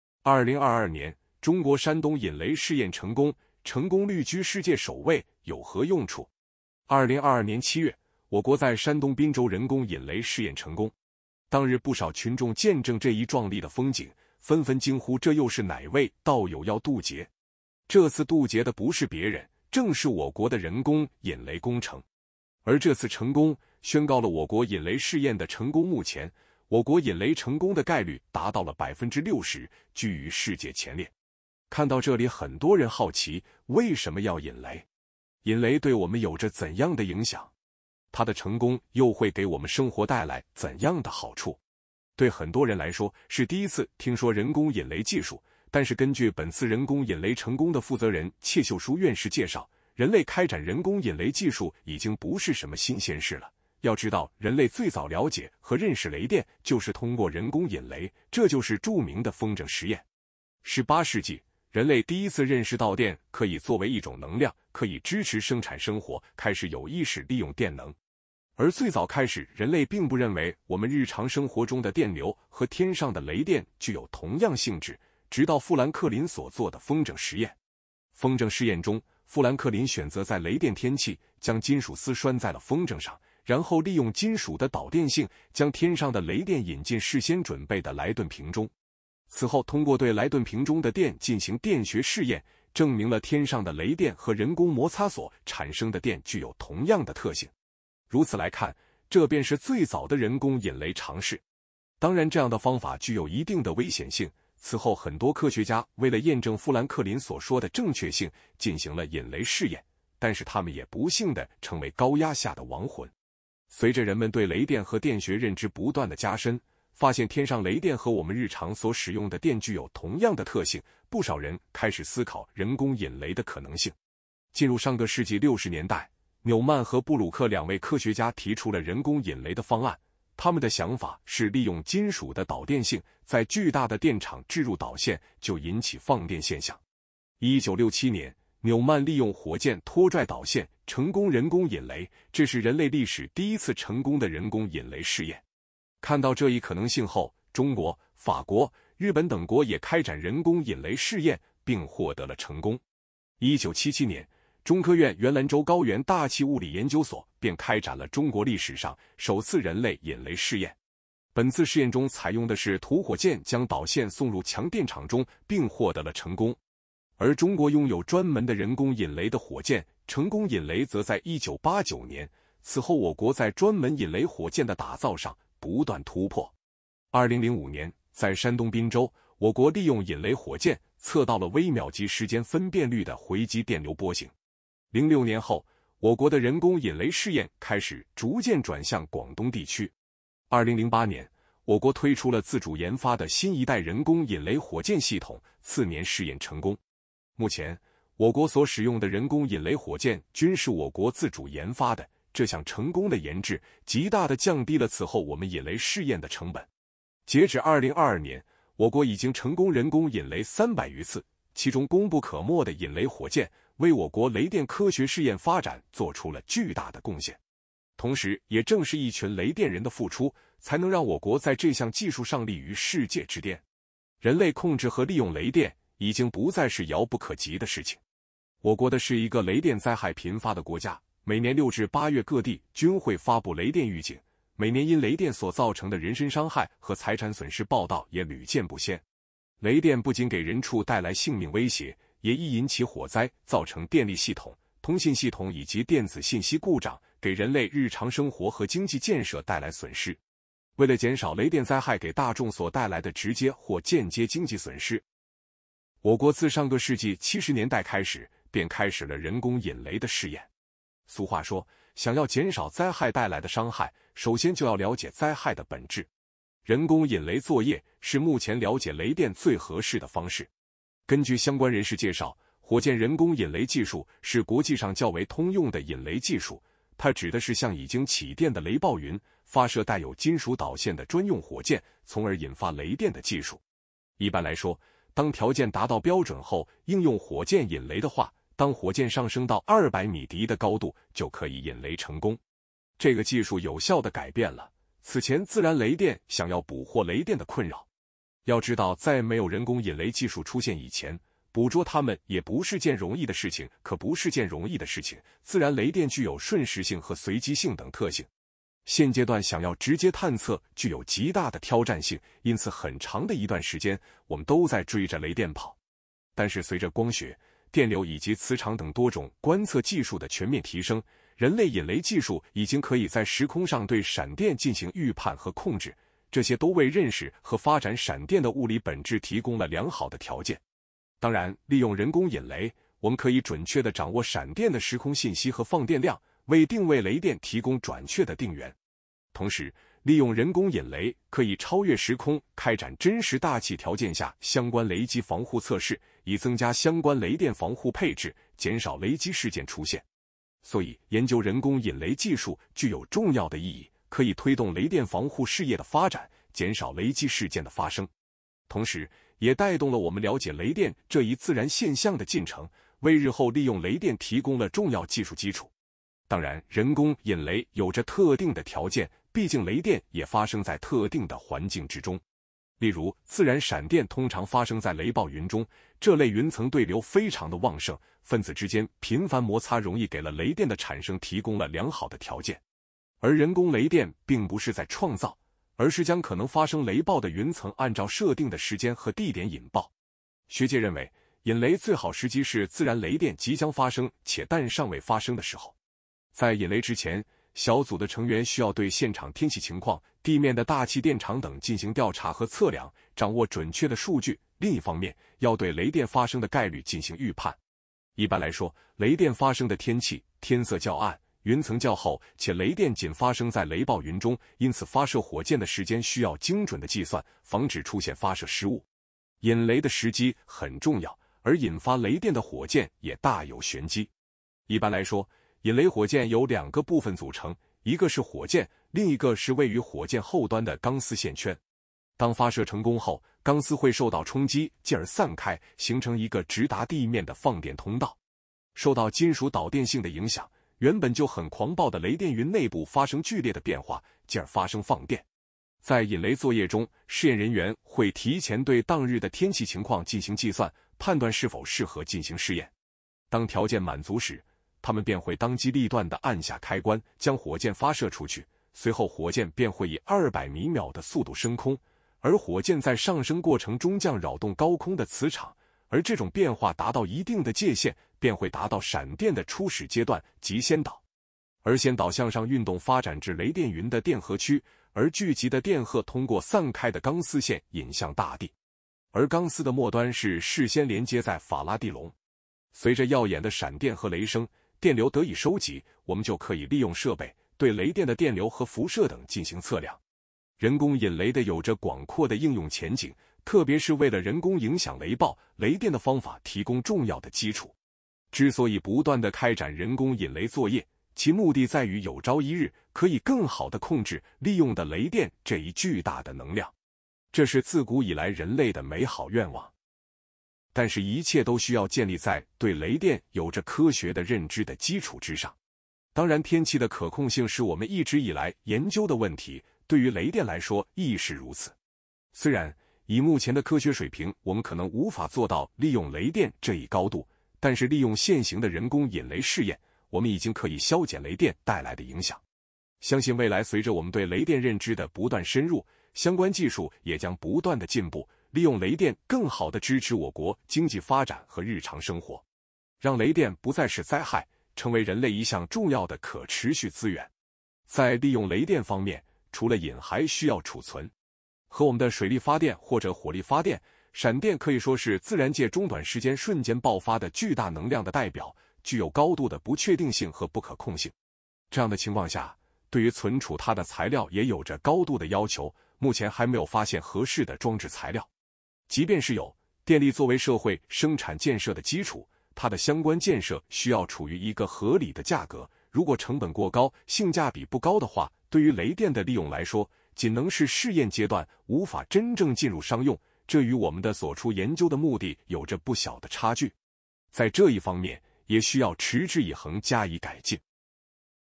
中国山东引雷试验成功，成功率居世界首位，用处细思极恐！
欢迎来到X科普局！从中国崛起看到中国基建的速度，再到中国制造的闻名！
中国，让我们倍感自豪！